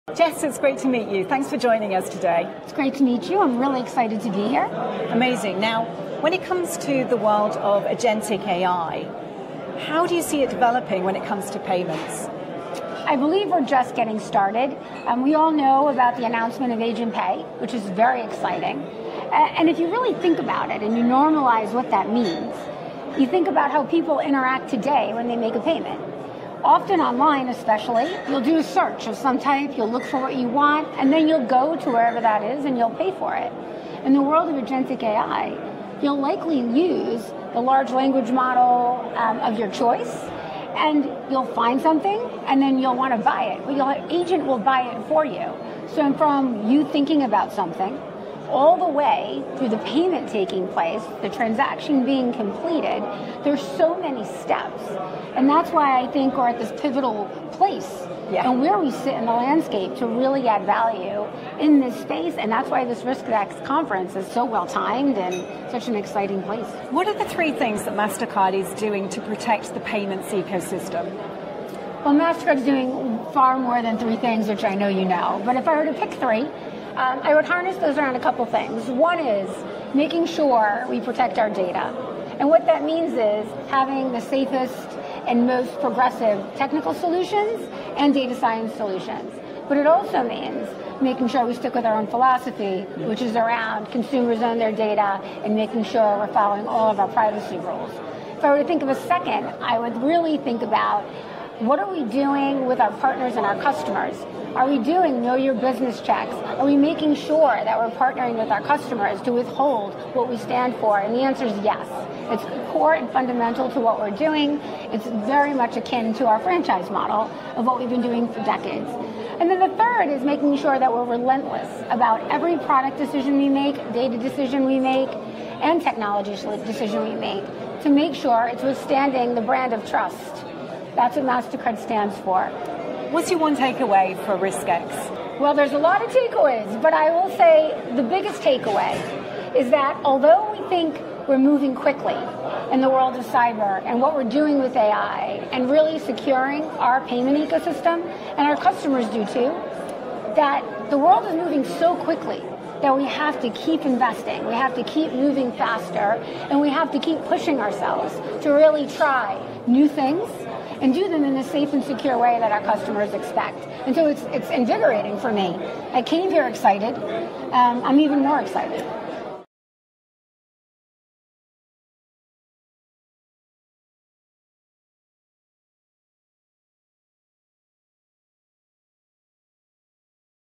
Agentic AI & the Future of Payments | Mastercard’s Jess Turner on Innovation, Trust & Security
At RiskX Rome 2025, we sat down with Jess Turner, Executive Vice President and Global Head of Open Finance at Mastercard, for a sharp, three-minute conversation about the intersection of AI, innovation, and security in payments.

In this exclusive discussion, Jess shares her perspective on:

🤖 Agentic AI in payments — how autonomous systems and generative intelligence are reshaping financial interactions, from real-time decisioning to smarter customer experiences.

🛡️ Three key initiatives Mastercard is driving to protect and strengthen the global payments ecosystem — balancing innovation, trust, and regulatory alignment.

💡 Her biggest takeaway from RiskX Rome — why collaboration and ecosystem trust are the foundation of the next wave of digital finance.

Whether you’re a fintech innovator, banking executive, or security leader, this short conversation delivers a clear, forward-looking view of how Mastercard is preparing for the agentic AI era while safeguarding financial integrity.

Watch to learn:

✅ How Agentic AI could redefine the world of payments
✅ Mastercard’s approach to protecting the financial ecosystem
✅ The mindset every leader should bring to open finance innovation

🎧 Recorded live at RiskX Rome 2025 — powered by Mastercard.